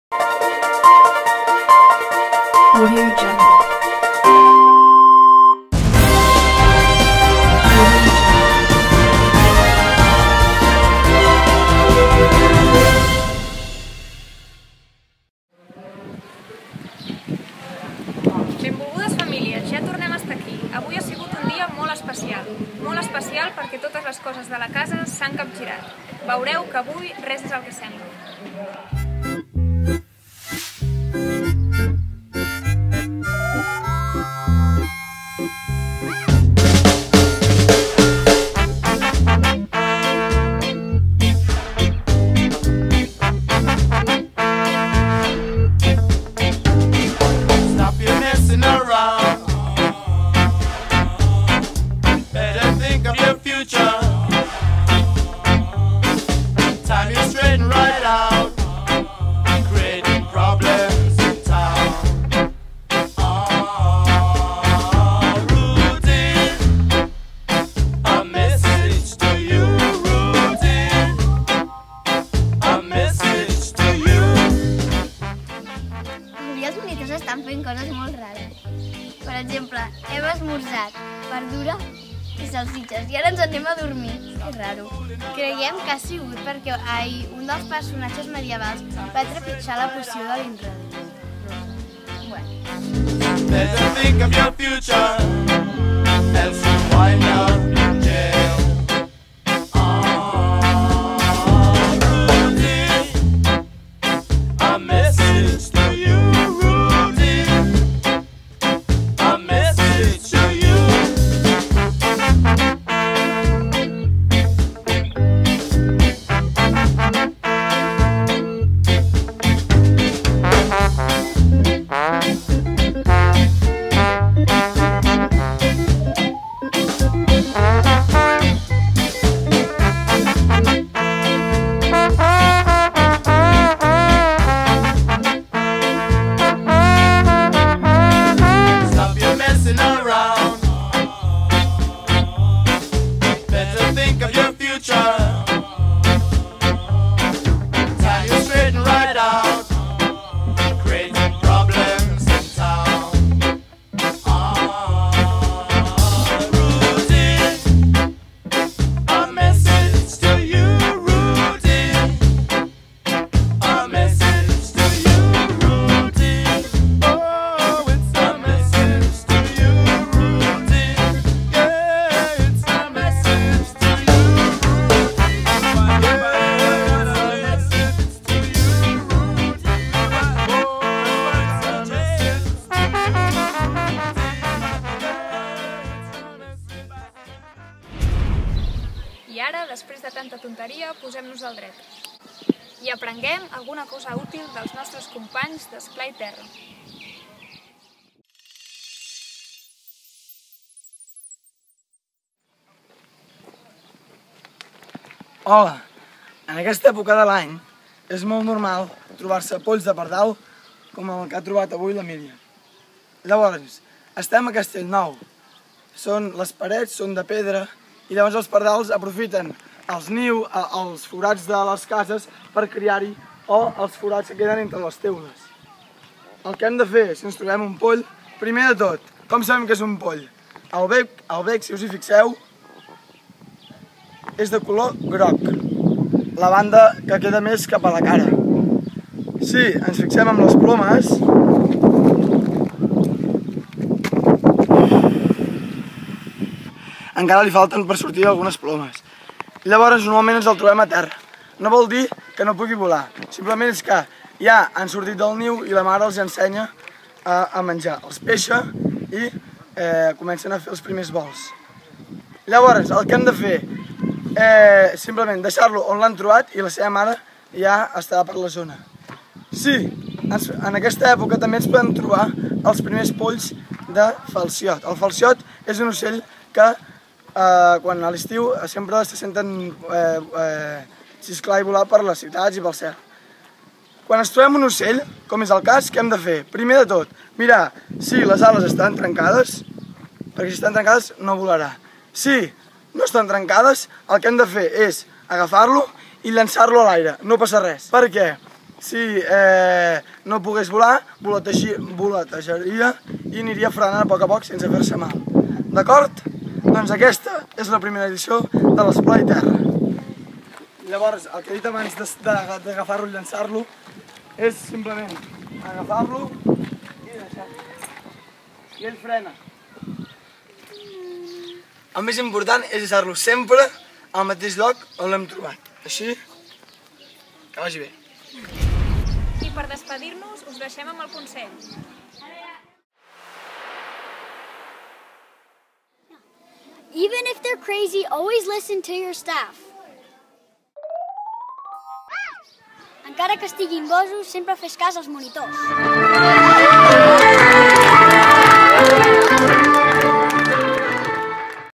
Castellnou News - 4t dia (15/07/2014)
Quart programa del noticiari de les colònies de l'Esplai 0'5 de 2014 a Dom Bosco (Castellnou de Bages)